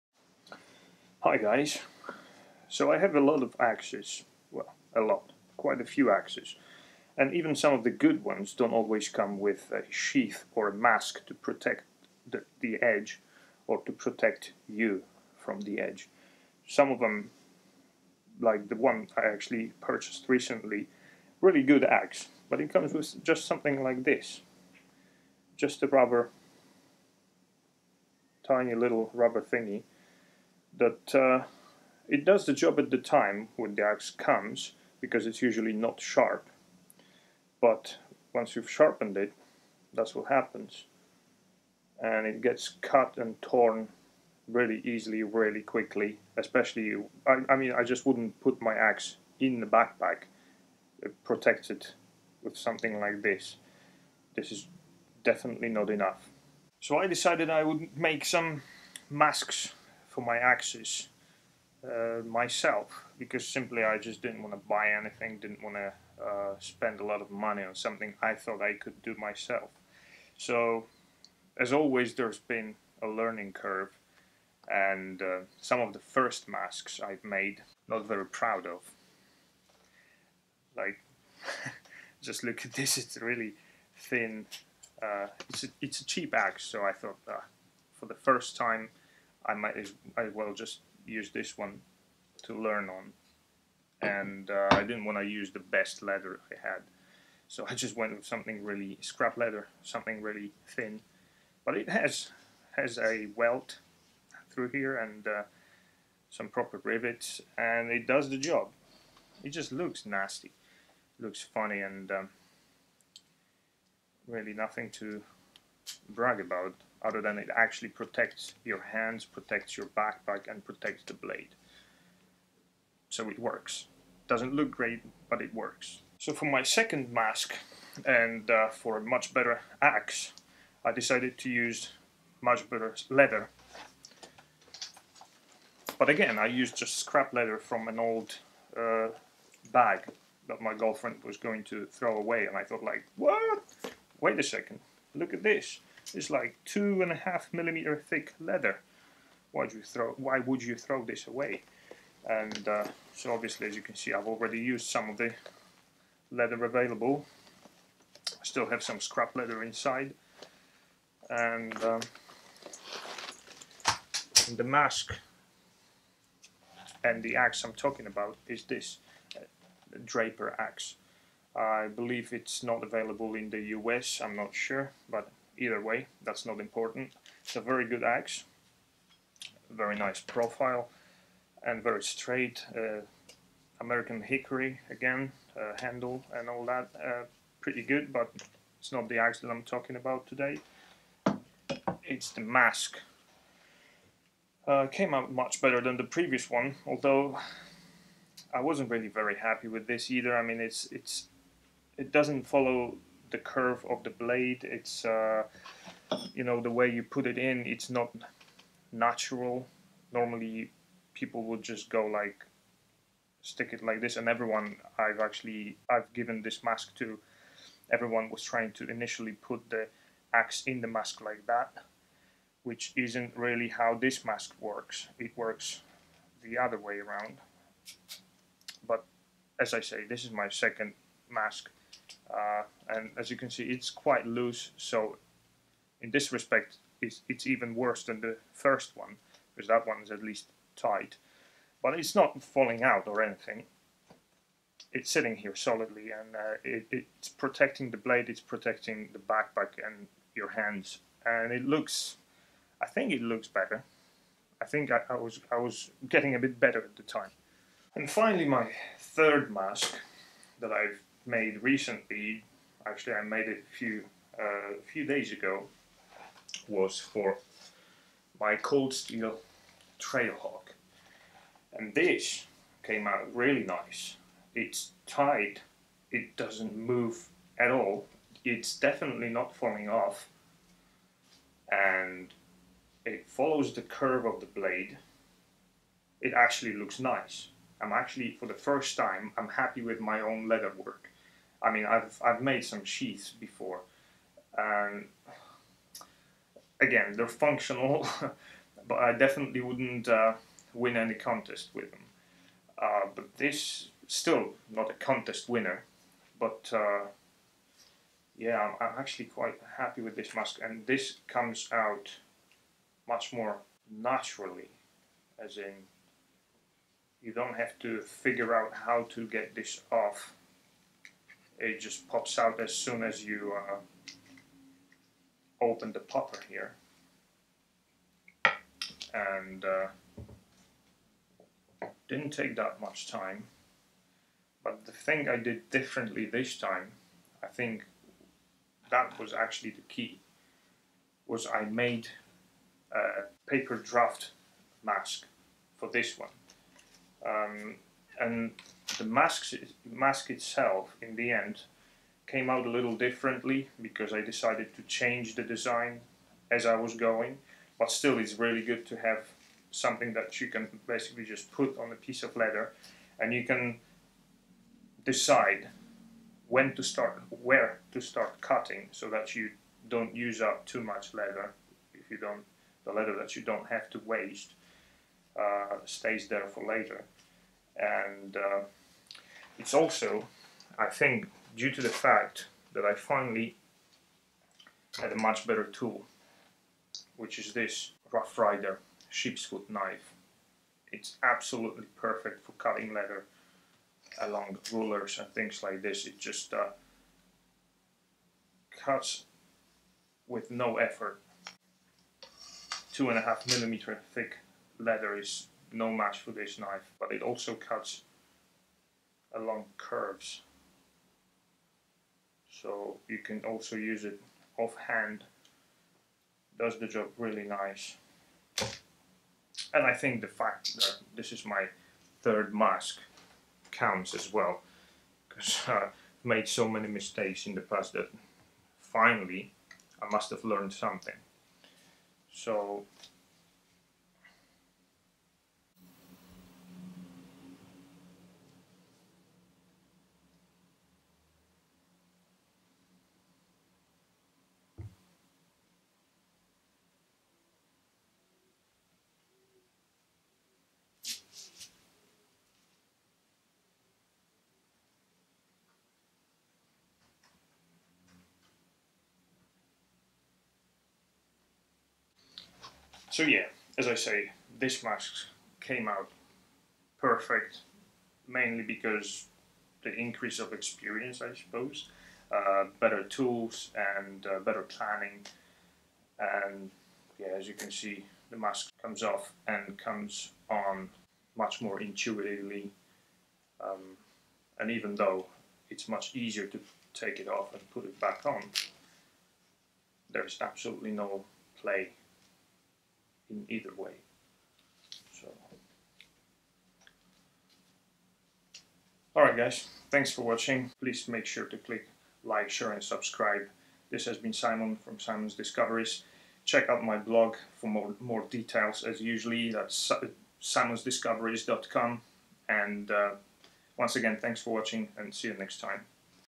Cold Steel Trail Hawk and Axe mask (Napisy) Skórzane Osłony na tomahawki i siekiery
Presenting some of my recent leatherwork I've made for a cheap stainless Silverline axe, a Draper hatchet and a Cold Steel Tomahawk (Trail Hawk). None of these came with a protective sheath or a mask, which meant I had to either make, or buy them separately if I was going to put them in a back pack and transport them safely and securely.

Sheaths or masks are really useful things and it's a shame most inexpensive axes or hatchets don't come with them. It's understandable though. If they did, they probably wouldn't be inexpensive anymore.

I noticed that it is possible to learn how to do the leatherwork yourself without spending any extra money. Especially if you have some of the materials needed at home. If you do not, it still won't be very expensive since all you need is a hammer, a few rivets, press studs with their punches, bolts and those little anvils, and scrap leather from an old bag, boots etc.

Of course, you most likely won't be pleased with your first attempt. Possibly even your second. I know I wasn't. But you still have to keep trying to get the experience and learn all those things you won't learn by watching videos or reading books. Hands-on experience my friend. Good luck and have fun :)

Jako, że wiele siekier i toporków sprzedawanych jest bez żadnej osłony ostrza, często zmuszeni jesteśmy wykonać je sami albo wydać więcej pieniędzy i dokupić je osobno, ewentualnie zlecić wykonanie komuś innemu. Zwłaszcza jeśli zamierzamy nasze siekierki, siekiery, topory i toporki nosić w plecaku, czy w ogóle gdzieś się z nimi wybierać poza domowe zacisze. Ostatnio wykonałem kilka takich osłon (masek) na ostrza i zauważyłem, że można się tego nauczyć i dość szybko poprawić. Mój pierwszy wytwór nie zachwycał. Drugi? no cóż... Za to za trzecim razem byłem już nieco bardziej zadowolony.